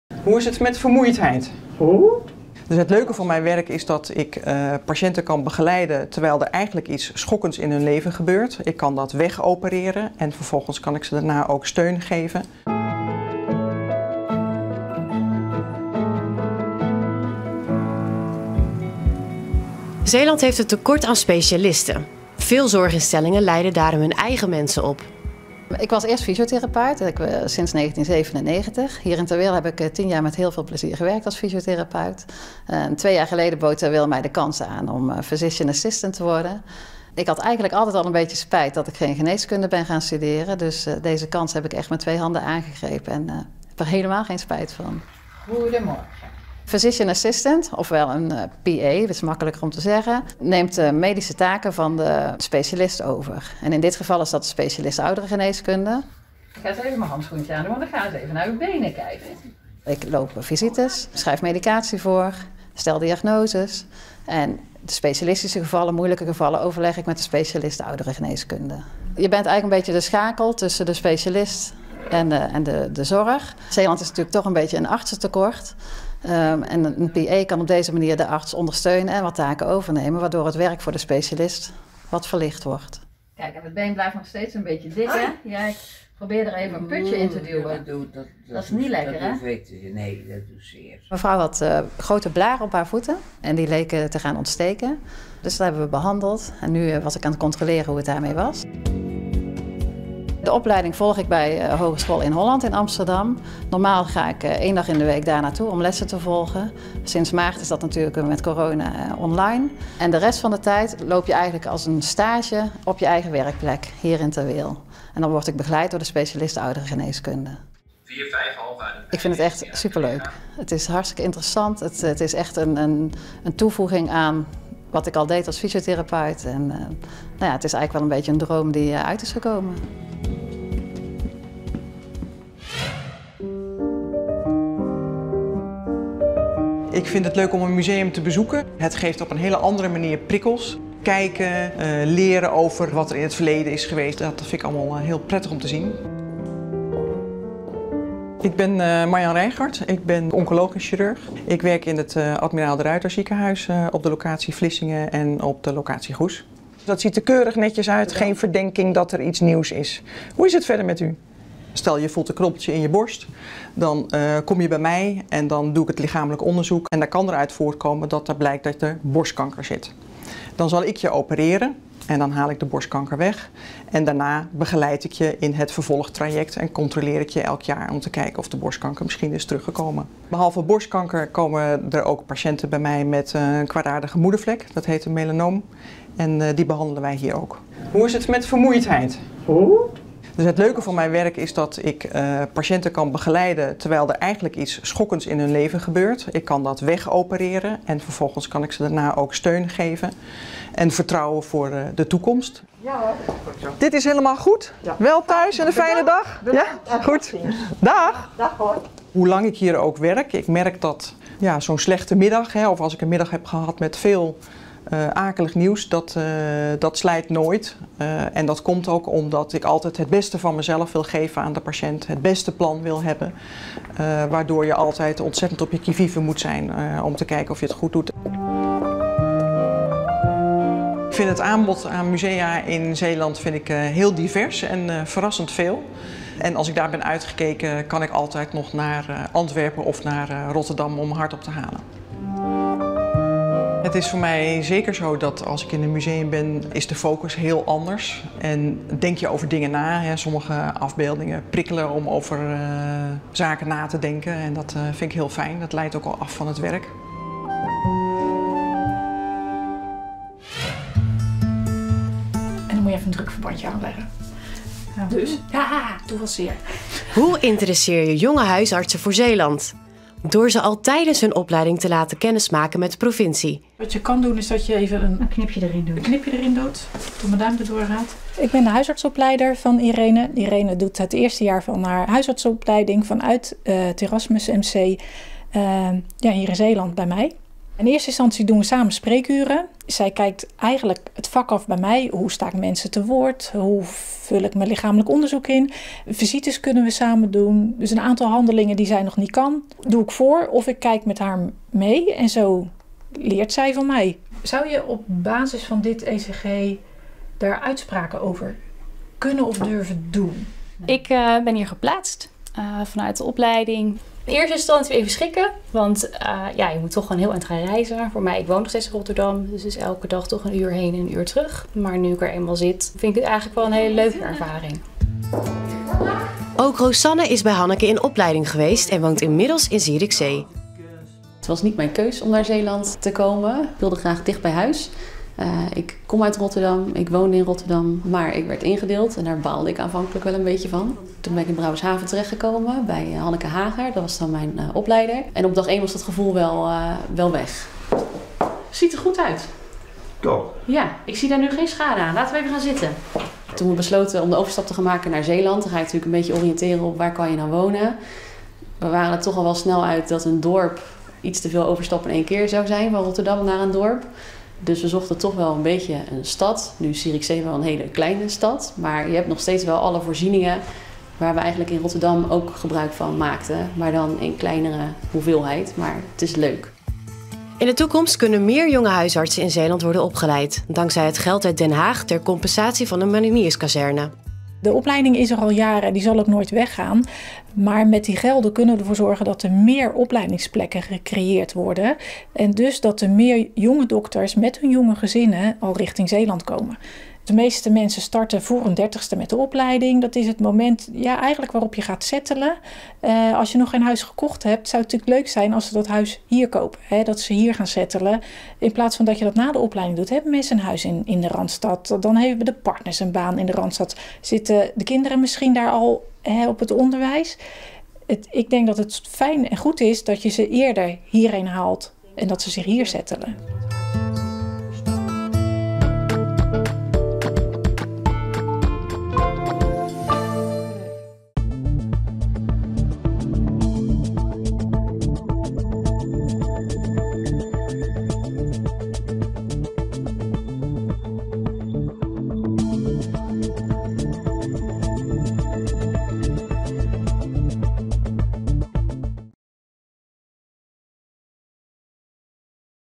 Zeeuwse Dokters, 25 november 2020
Opleidingen in Zeeland moeten het tekort aan artsen en specialisten tegengaan en cultuur snuiven in Zeeland met medisch specialist Marjan.